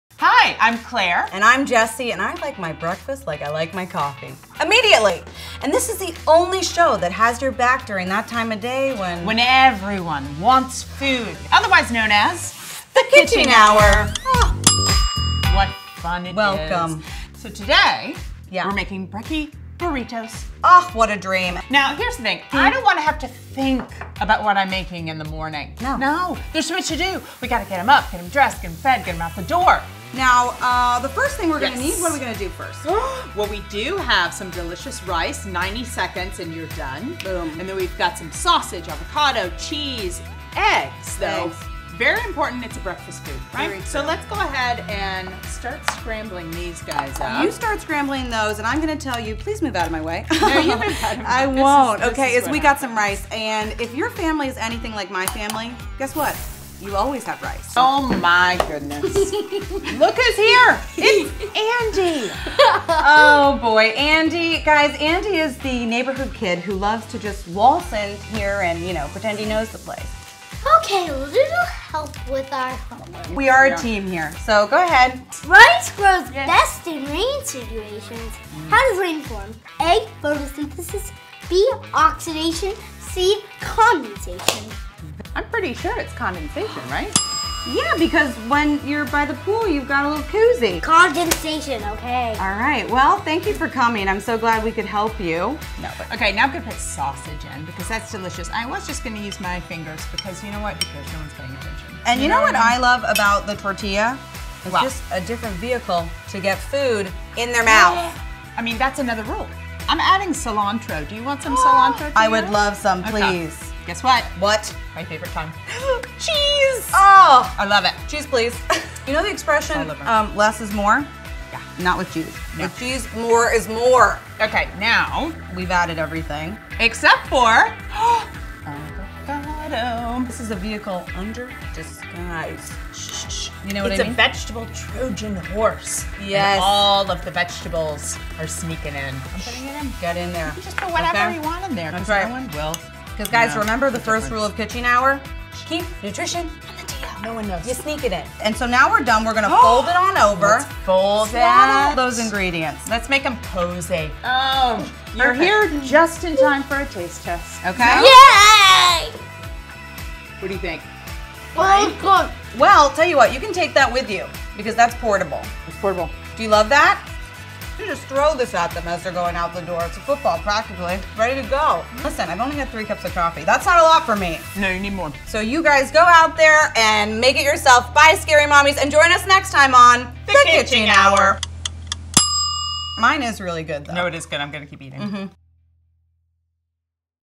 A Brekkie Burrito That’s Quick, Easy, and Delicious | The Kitching Hour | Scary Mommy
Busy mornings don’t mean you can’t enjoy a hot breakfast that the family will love. These burritos take minutes to make and are as versatile as you want them to be: More cheese? Heck yeah. Want to include leftovers? Go for it. The best part is how they can be eaten on the go. So even if they’re late, you can hand them breakfast as they walk out the door.